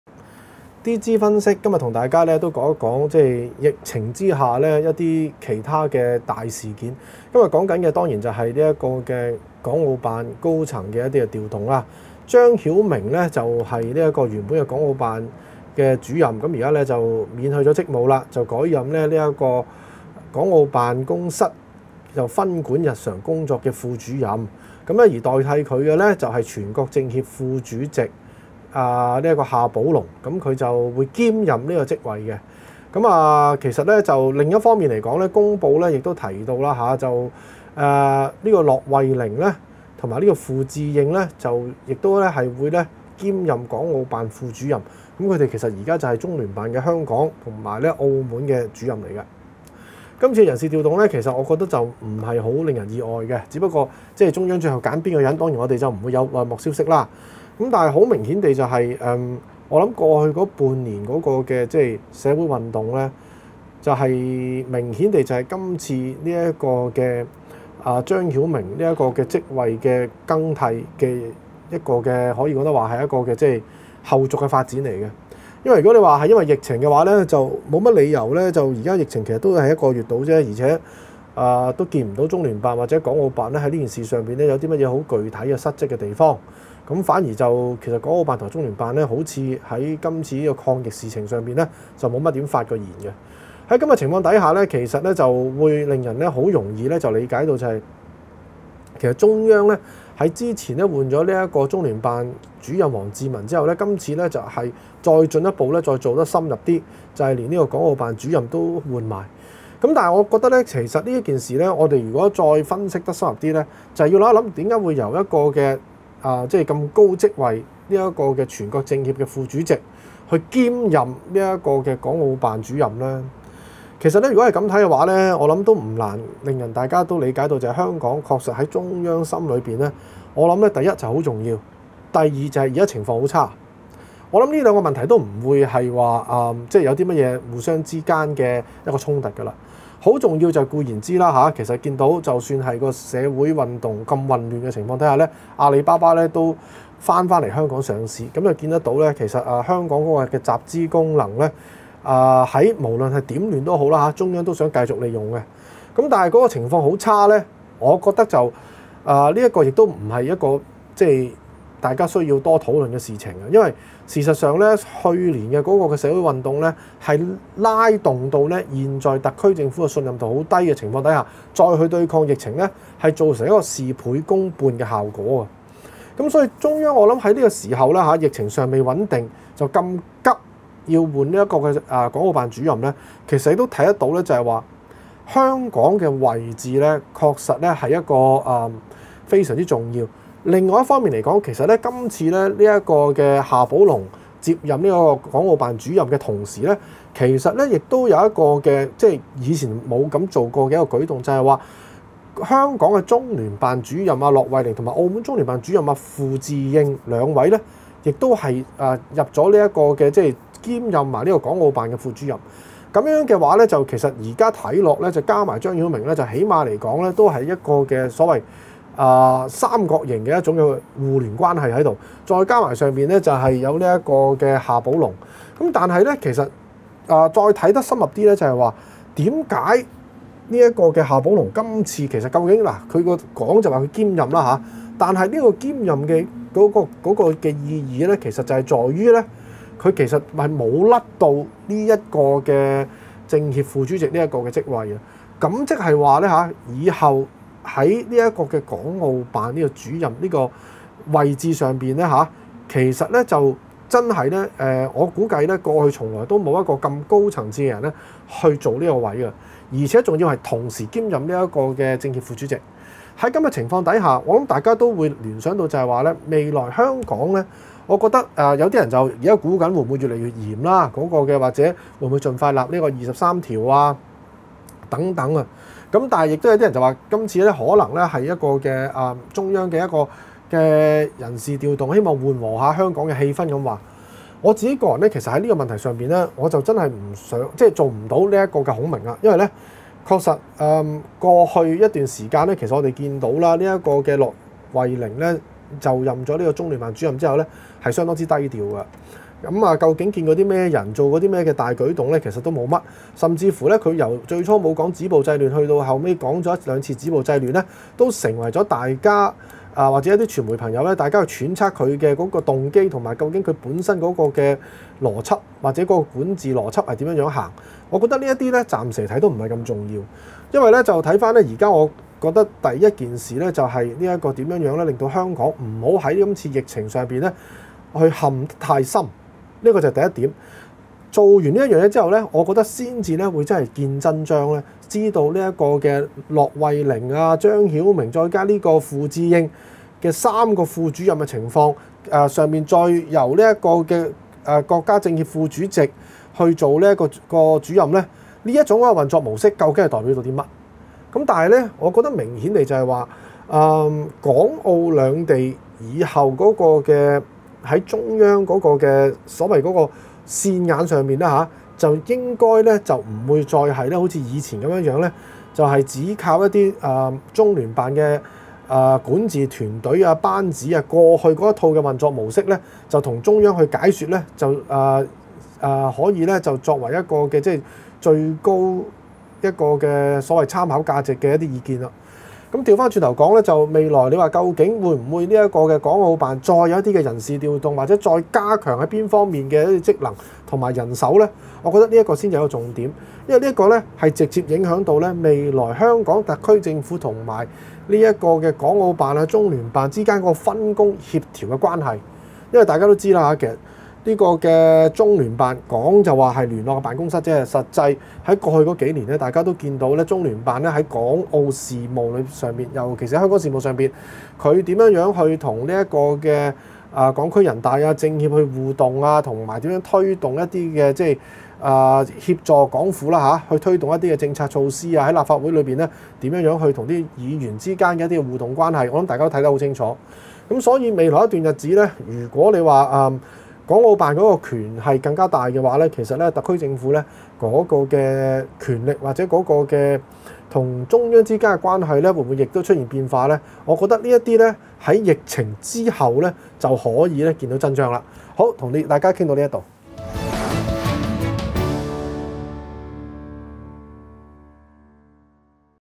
【D之分析】港澳辦變陣還有後著
香港備受肺炎疫症威脅下，港澳辦高層出現人事變動。國務院公布，免去國務院港澳事務辦公室主任張曉明職務，改任港澳辦分管日常工作的副主任。而港澳辦主任一職，將由政協副主席夏寶龍接替，香港中聯辦主任駱惠寧和澳門中聯辦主任傅自應則兼任港澳辦副主任。
今次人事調動，是否反映中央對港澳事務的另一種取態？且看Danny分析。